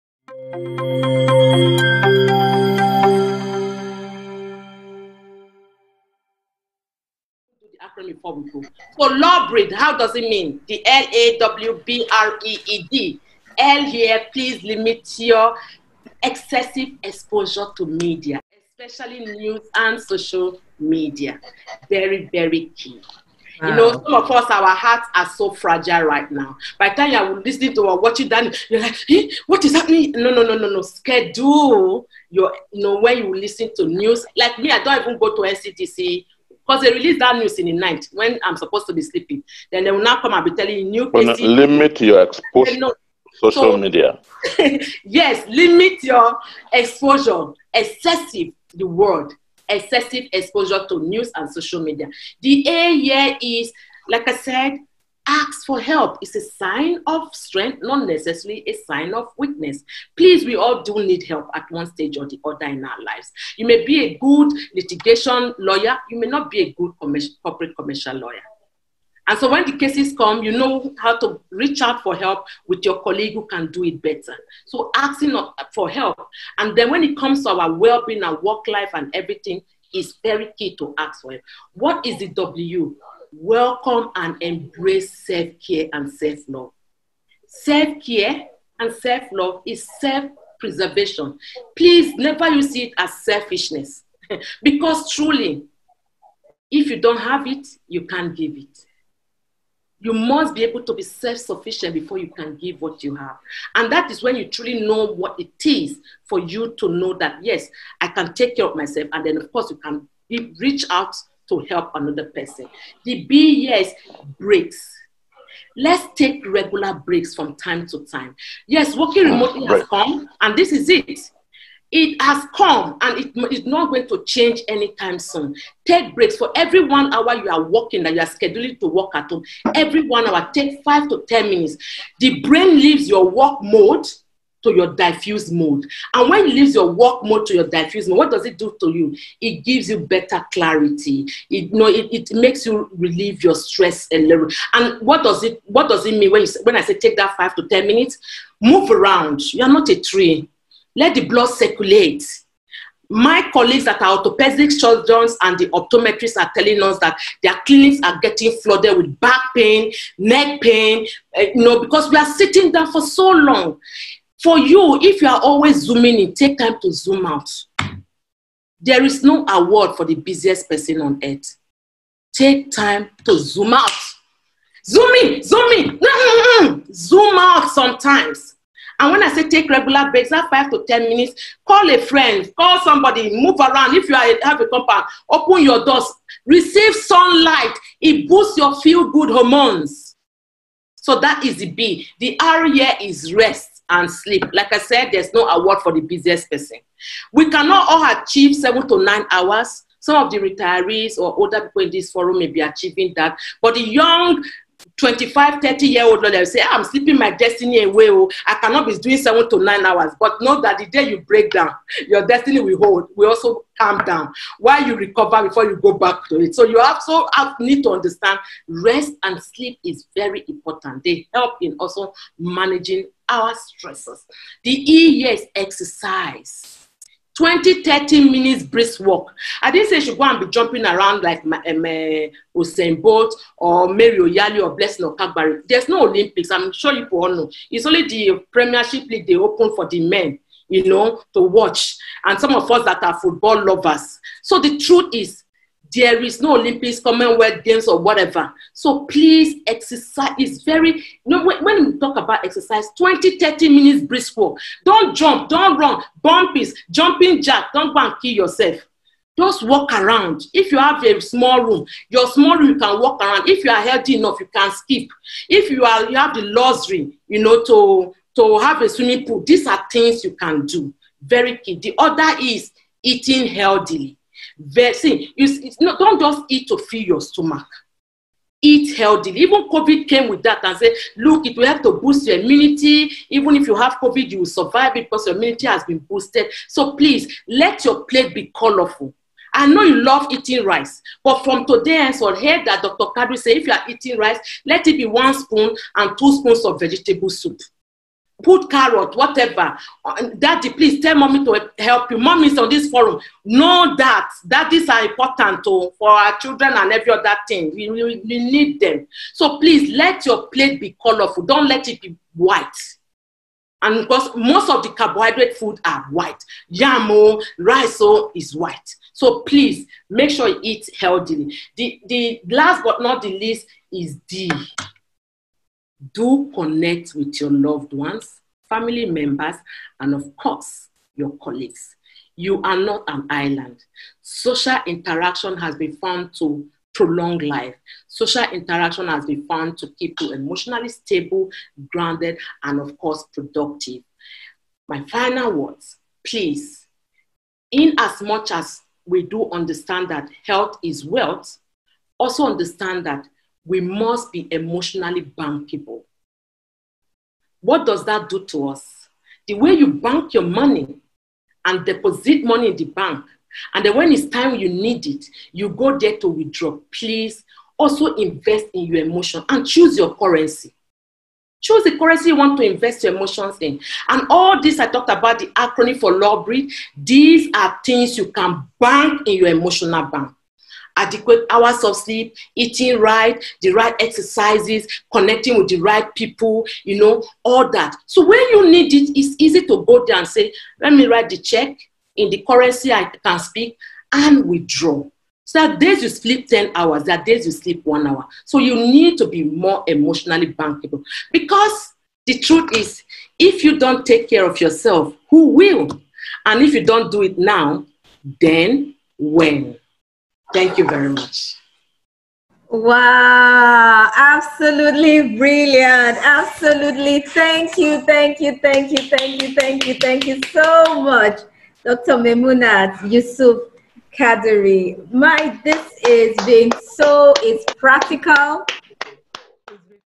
DR MAYMUNAH KADIRI AT LAWBREED WEBINAR 2
DR. MAYMUNAH YUSUF KADIRI, Consultant Neuro Psychiatrist, C.E.O Pinnacle Medical Services Speaking at LAWBREED ACADEMY’S Webinar on Lawyers Wellbeing with the LAWBREED Acronym:

A   - Ask for Help
W  - Welcome and Embrace self-care and self-love
B   - Breaks, Take regular breaks
R   - Rest and Sleep
E   - Exercise
E   - Eating healthy
D  - Do connect with your loved ones, Families, and Colleagues

The Webinar was tagged; MONEY MATTERS AND THE LAWYER’S WELL- BEING series 2.  It was a conversation to further enlighten lawyers on how to manage/handle some key issues of life, such as creating and managing wealth and doing so living a balanced life.

It was loaded, impactful, amazing, insightful.

LAWBREED .EQUIPPING YOU FOR GREATER SUCCESS.